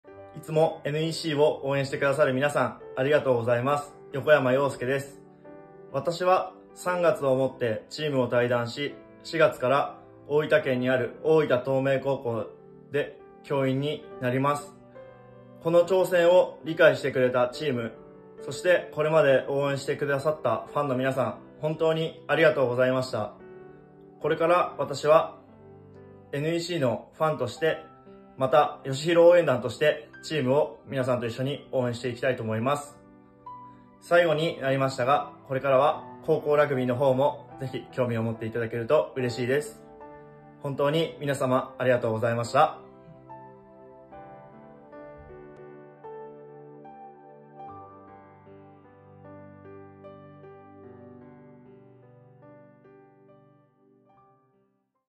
【退団選手コメント】横山 陽介
2021年度をもって、NECグリーンロケッツ東葛を退団する横山陽介さんより、皆様へメッセージです。